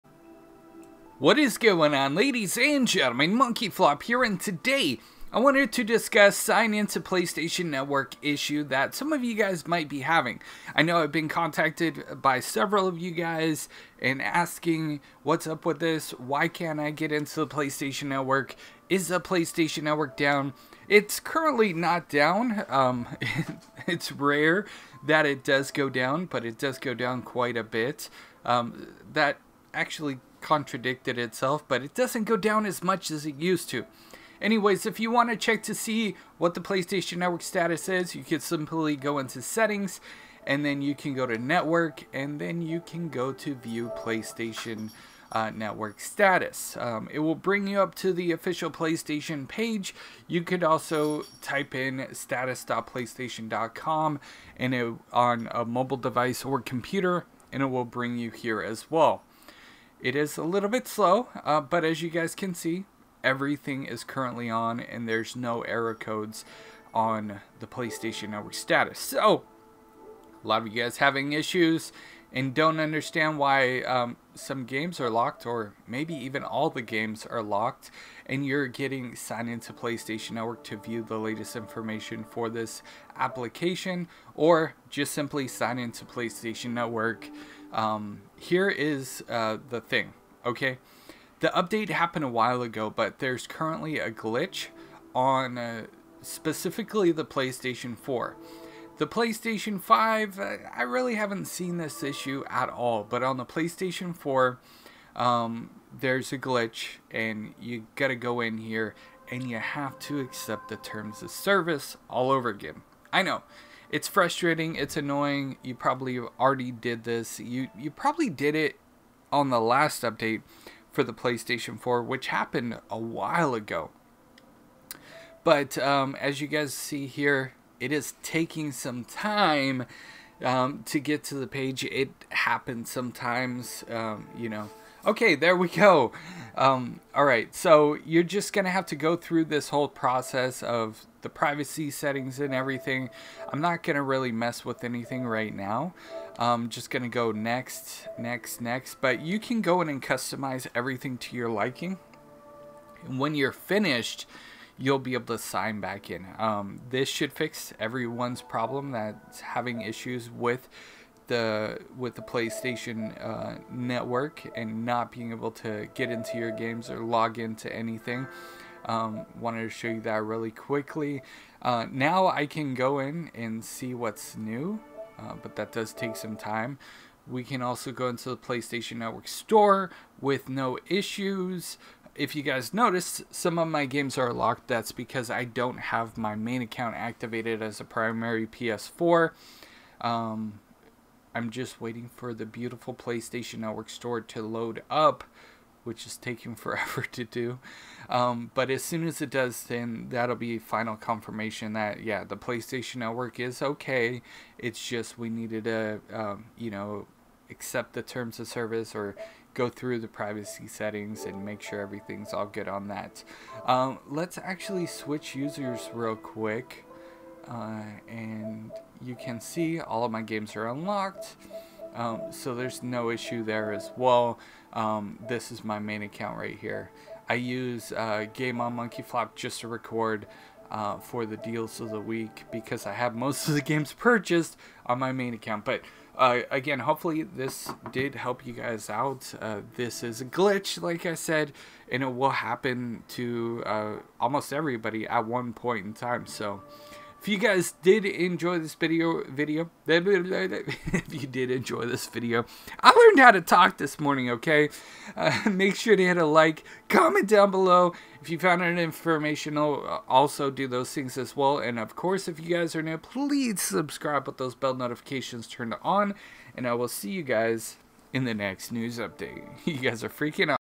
Cant Sign Into PSN Problem on PS4 & How to FIX
Cannot Sign Into PSN Problem on PS4. This issues with signing in to PSN on PS4 has effected a lot of gamers and some of you asked how to fix this problem so I created this video. It is a ps4 software glitch after the latest system software update. I hope it fixes some of your issues!

Follow @MonkeyFlop on Twitter, Twitch, Facebook, or just search MonkeyFlop